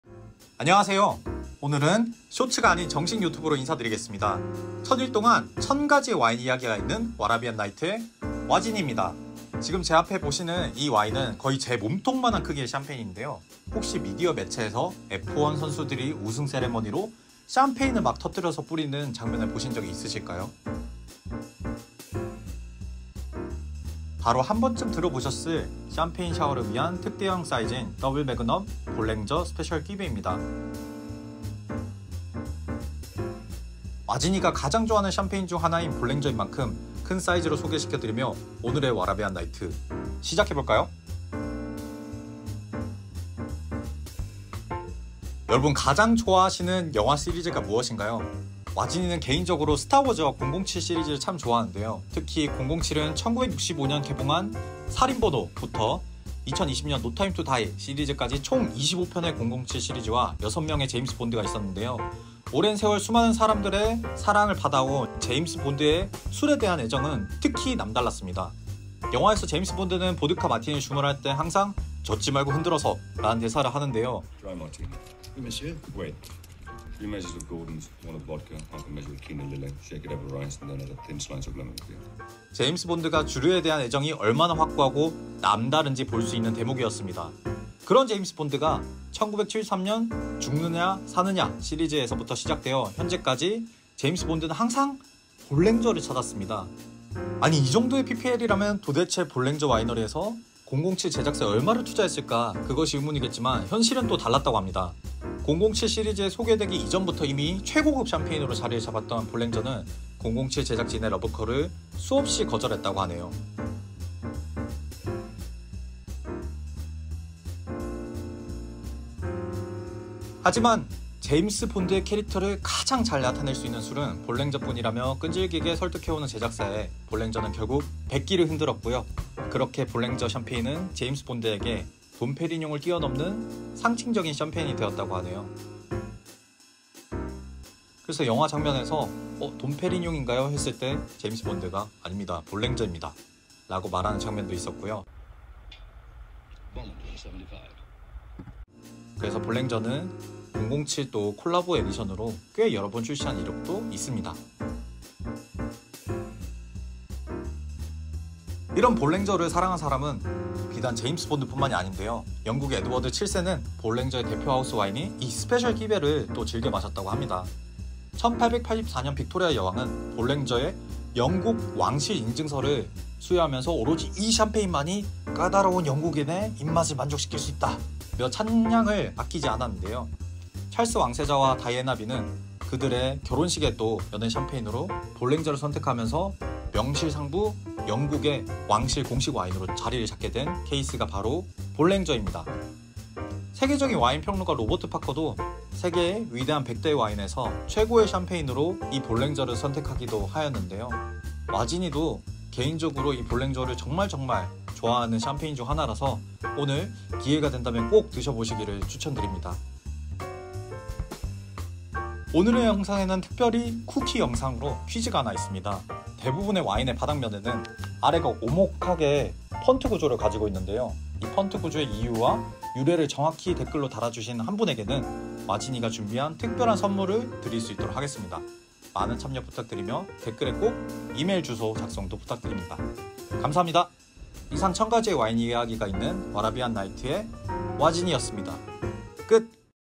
[🌟 이벤트 ] : 007 제임스본드가 본드걸보다 사랑한 샴페인 🍾
와라비안나이트에 대한 사랑에 보답하고자 이번에는 이벤트를 갖고 왔습니다.
즐겁게 시청 해 주시고 많은 참여 부탁드립니다.
2023년 8월 31일까지 입니다!

빅토리아 여왕이 사랑한 007의 샴페인, 볼랭져